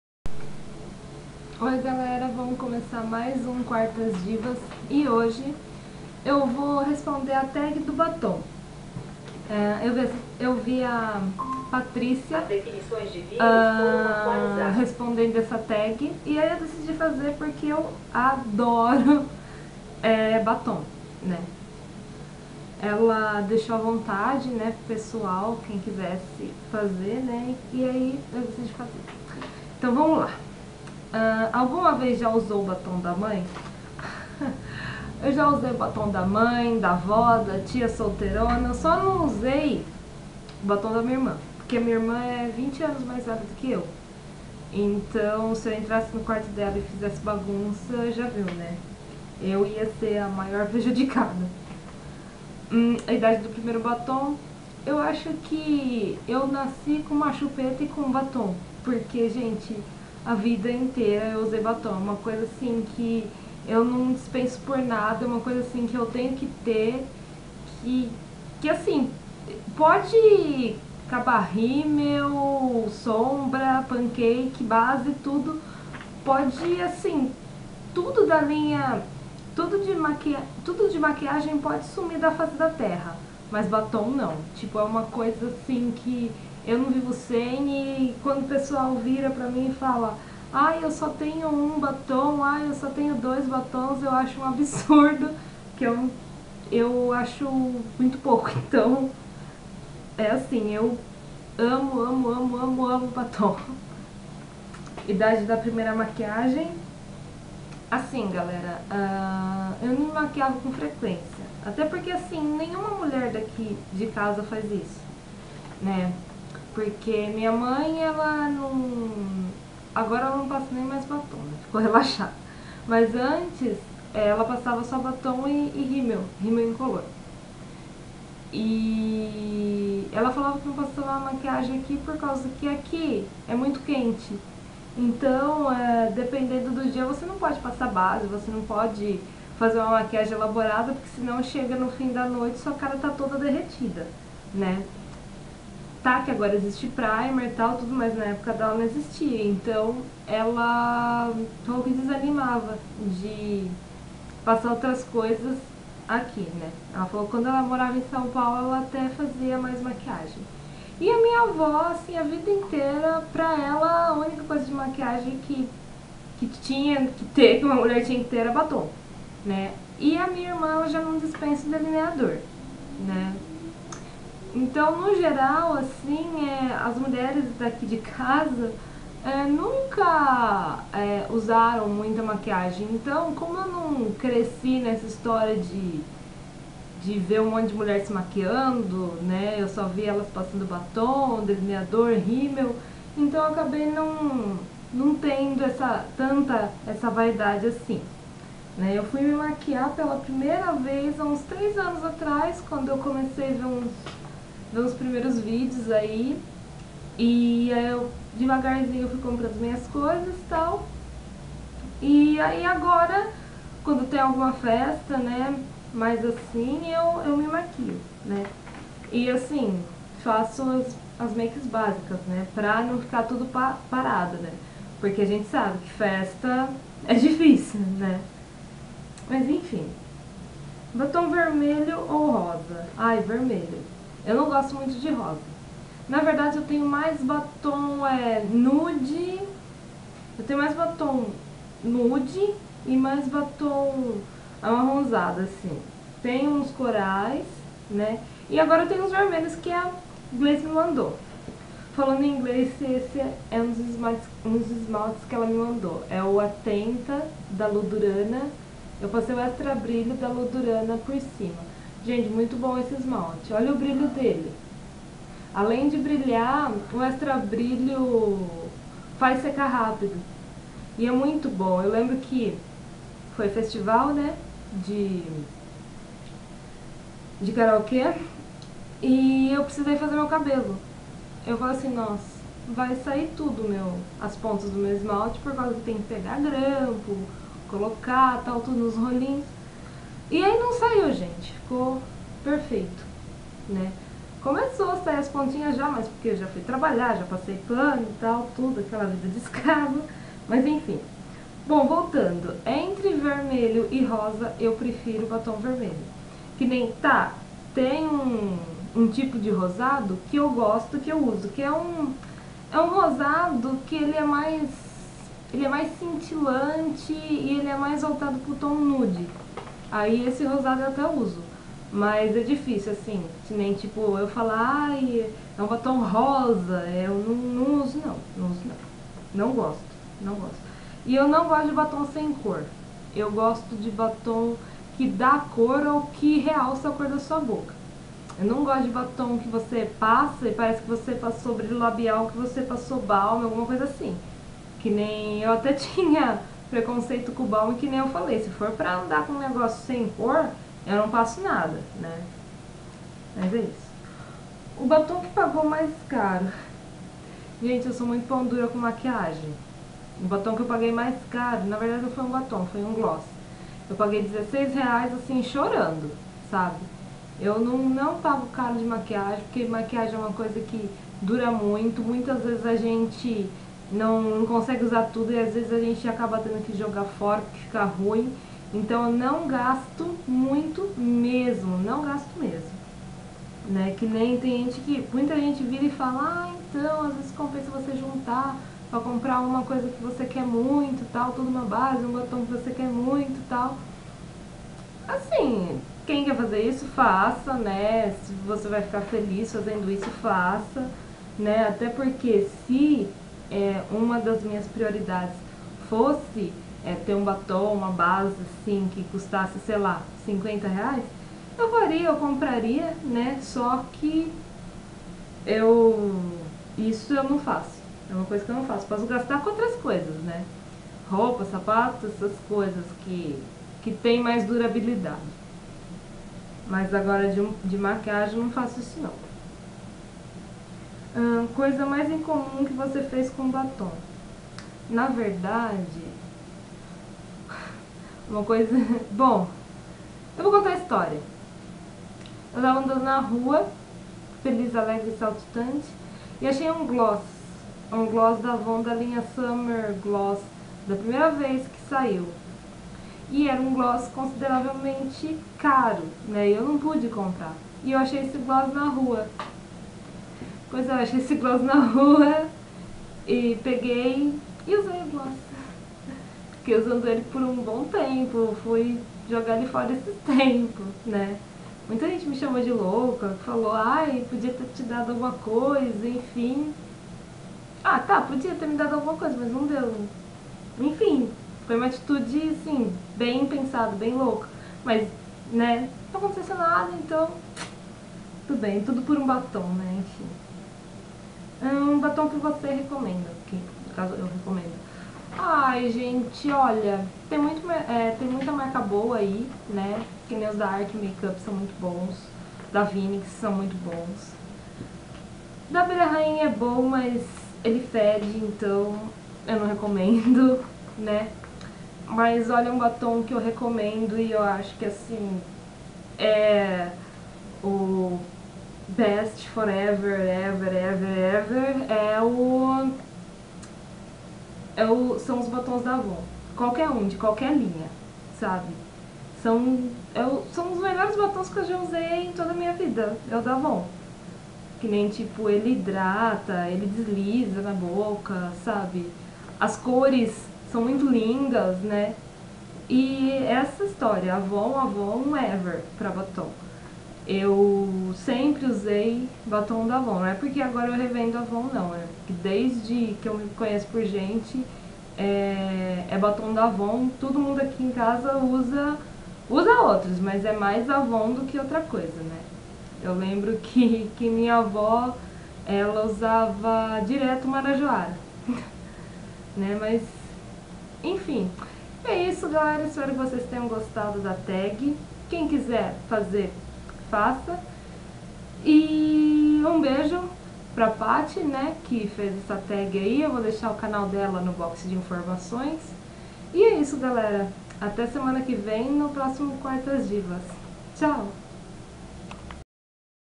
37# Quartas Divas - Tag do Batom
Olá galera, vi a paty do canal: honeybeesp respondendo essa tag e adorei, espero que gostem
bjs
me sigam no twitter: @ary_chan